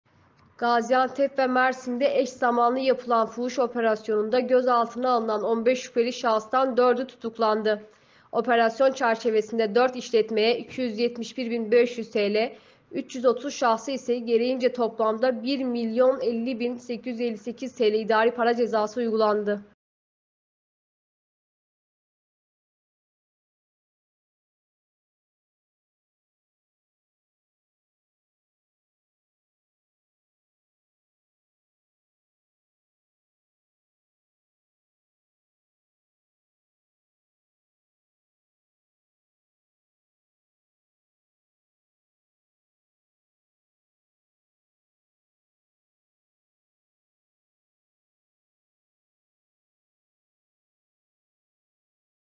Fuhuş operasyonunda 15 gözaltı
Operasyon sonucunda 330 şahsa 1 milyon 50 bin 858 TL idari para cezası uygulandı